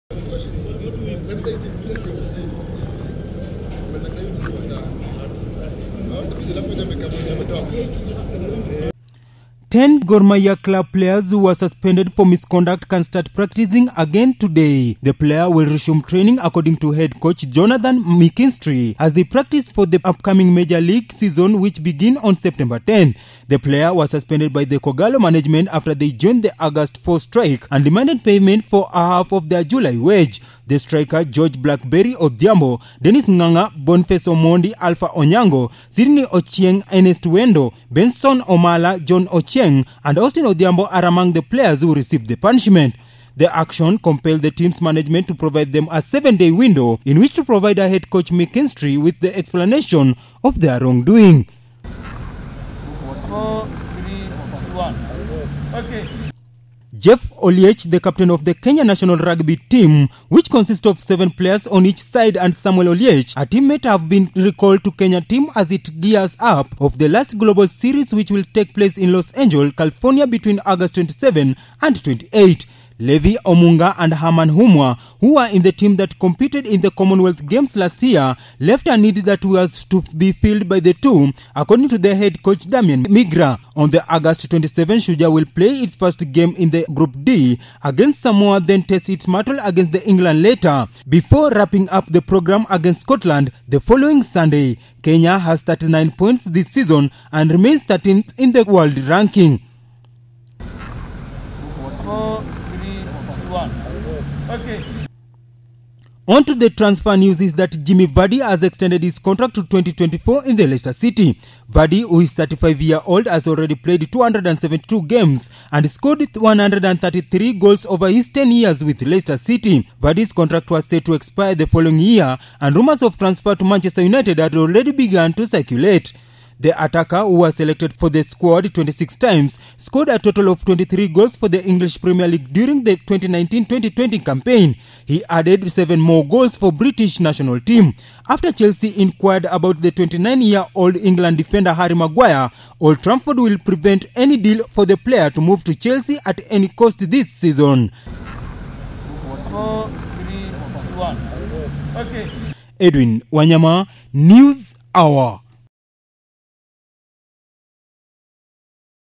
Gor back to training
Ten Gor Mahia club players who were suspended for misconduct can start practicing again today.

The players will resume training, according to head coach Jonathan McKinistry, as they prepare for the upcoming major league season, which begins on September 10.

The players were suspended by the K'Ogalo management after they joined the August 4 strike and demanded payment of half of their July wage.

Striker George "Blackberry" Odhiambo, Dennis Ng'ang'a, Boniface Omondi, Alpha Onyango, Sydney Ochieng, Ernest Wendo, Benson Omalla, John Ochieng, and Austin Odhiambo are among the players who received punishment.

The action compelled the team's management to provide them a seven-day window in which to provide head coach McKinstry with an explanation of the wrongdoing.

Jeff Oluoch, the captain of the Kenyan national rugby team, which consists of seven players on each side, and Samuel Oliech, a teammate, have been recalled to the Kenyan team as it gears up for the last global series, which will take place in Los Angeles, California, between August 27 and 28.

Levi Amunga and Herman Humwa, who were in the team that competed in the Commonwealth Games last year, left a need that was filled by the two, according to head coach Damian MGrath

On August 27, Shujaa will play its first game in group D against Samoa, then test its mettle against England later , before wrapping up the program against Scotland the following Sunday. Kenya has 39 points this season and remains 13th in the world rankings.

Onto transfer news

Jamie Vardy, has extended his contract to 2024 in Leicester city.

Vardy, who is 35 years old, has already played 272 games and scored 133 goals over his ten years with Leicester.

Vardy's contract was set to expire the following year, and rumors of a transfer to Manchester United had already begun to circulate.

The attacker, who was selected for the squad 26 times, scored a total of 23 goals for the English Premier League during the 2019–2020 campaign. He added seven more goals for the British national team.

After Chelsea inquired about the 29-year-old England defender Harry Maguire, Old Trafford will prevent any deal for the player to move to Chelsea at any cost this season.